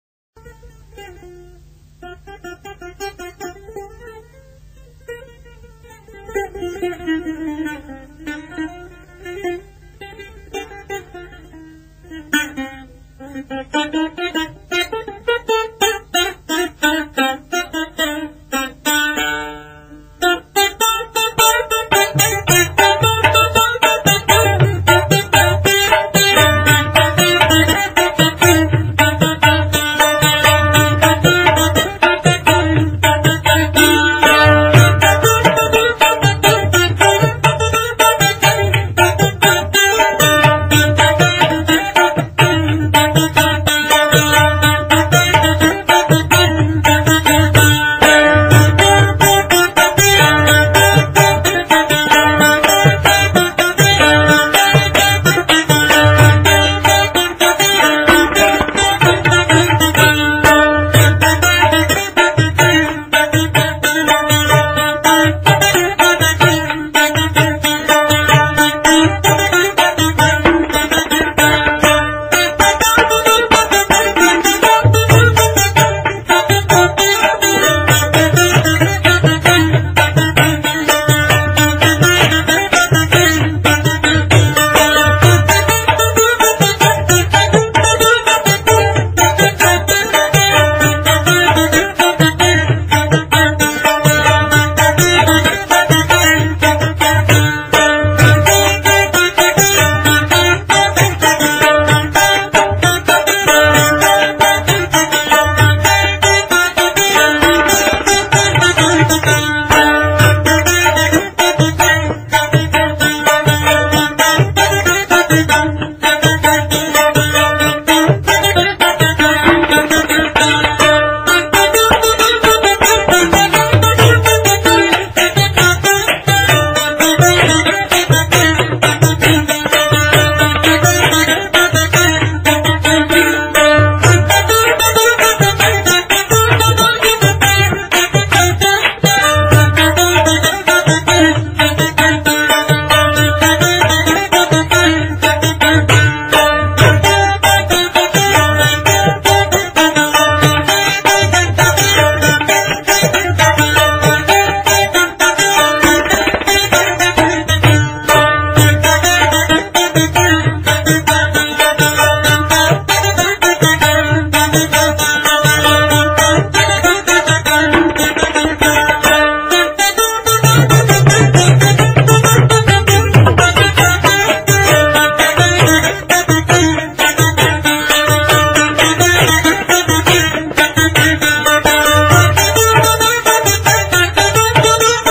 Behboodi village january 2009, Pictures views, rabab music remix, Pashto rabab, Rabab Music
Behboodi village january 2009, Pictures views, my visit to village behboodi in 2009,

rabab music remix,